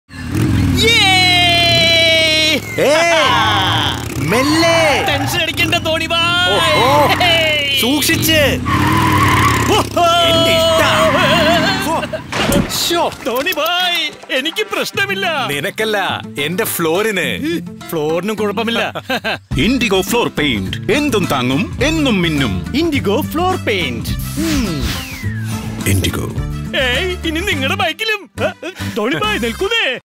Malayalam - Floor Coat Emulsion 30 Sec
Beautify your house from the inside as well as from the outside. The all-new Floor Coat Emulsion from Indigo Paints will not only protect your floors from abrasions but will keep it shiny and new for years to come. The Floor Coat Emulsion protects your flooring from algal or fungal attack. It is suitable for application on terrace floor tiles, cement tiles affixed to driveways and walkways, or on any plain cement surface. The paint not only provides an aesthetic glossy finish, but is also resistant to abrasion from human and vehicular traffic, sun, rain, and fungal attack.